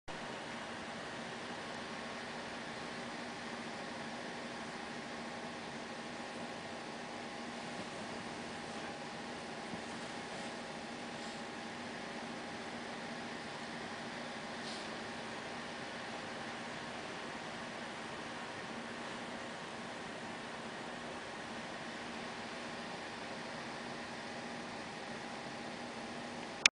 The Babies in Pennys Belly
Penny belly 5 days before the puppies were born